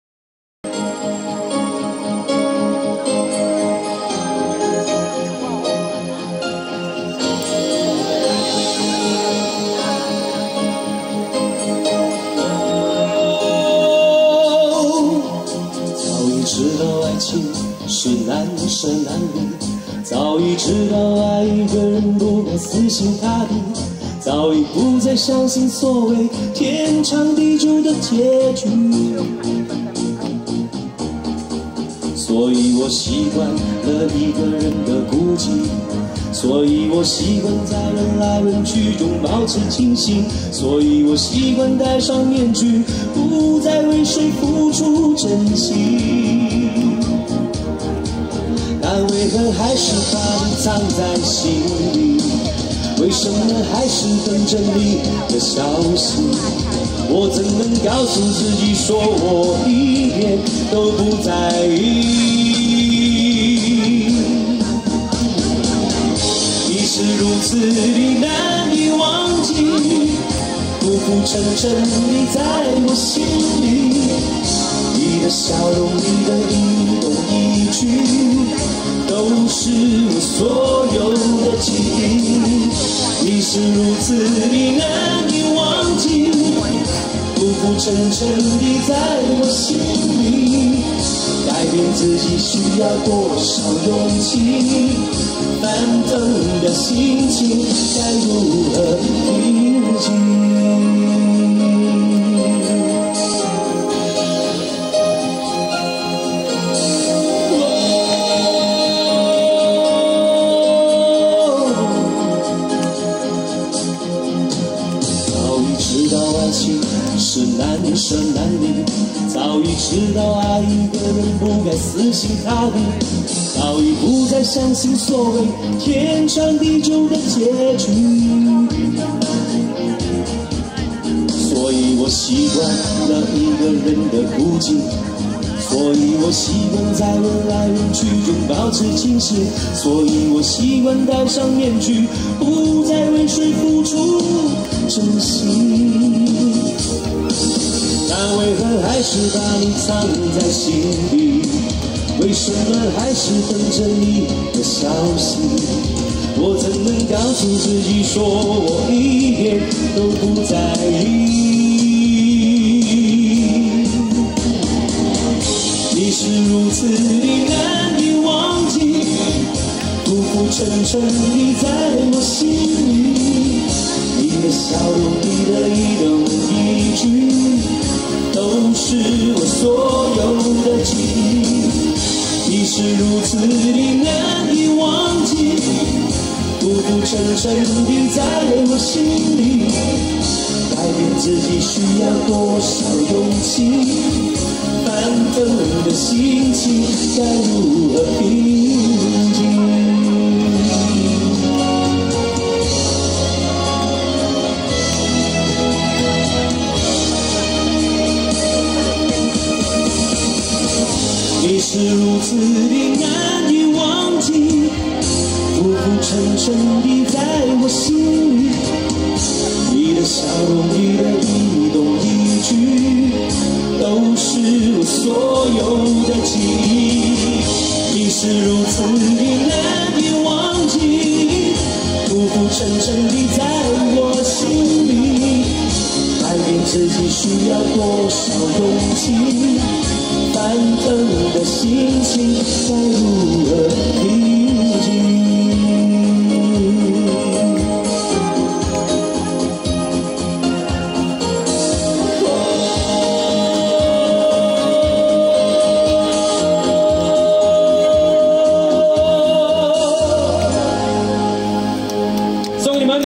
Jason Chung sang a song.
Programme: Fun in Diversity Night
Date: 23/01/2023 (大年初二）
Location: River Angbao 2023 @ The Garden By The Bay